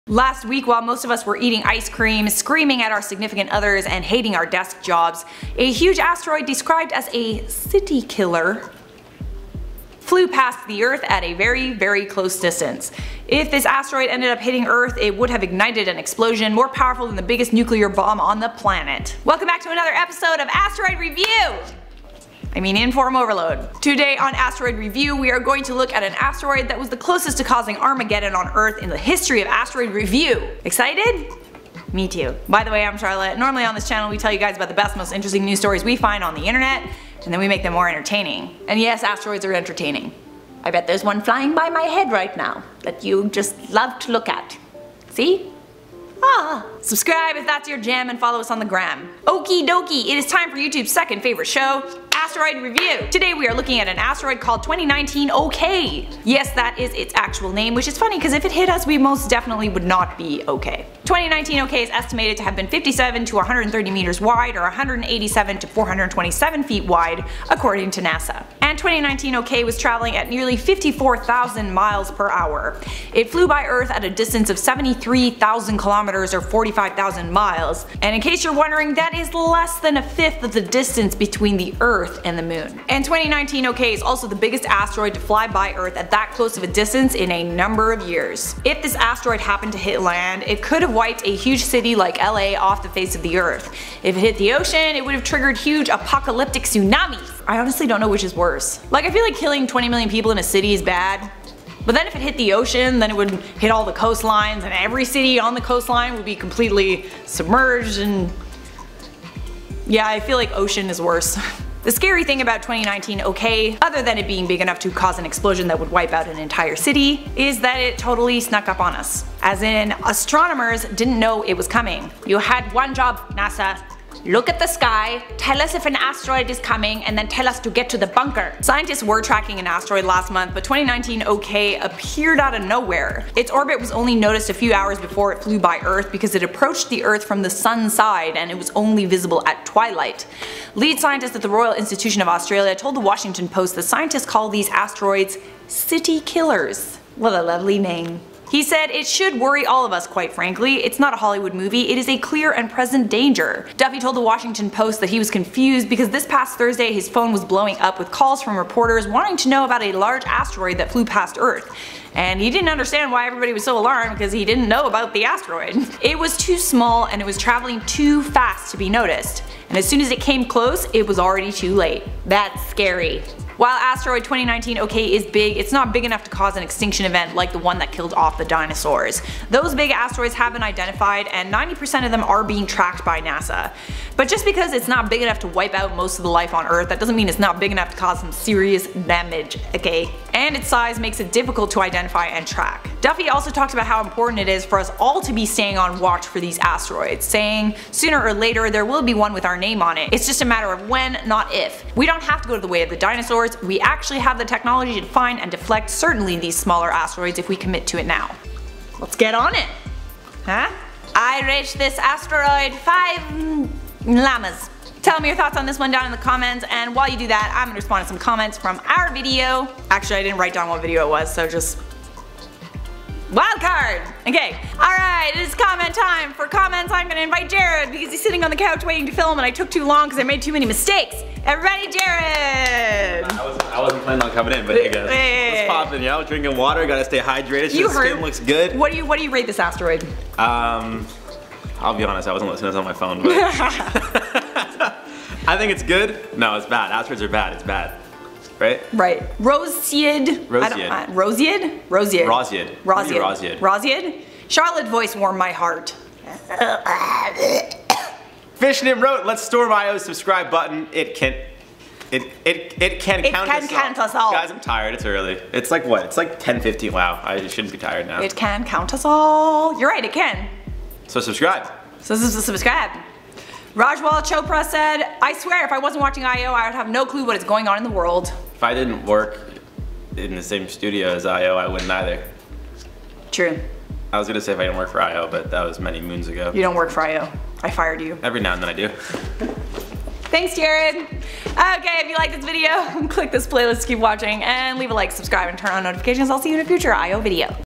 Huge “City Killer” Asteroid Almost Hit Earth
Last week, a huge asteroid described as a city killer flew past earth at a very close distance.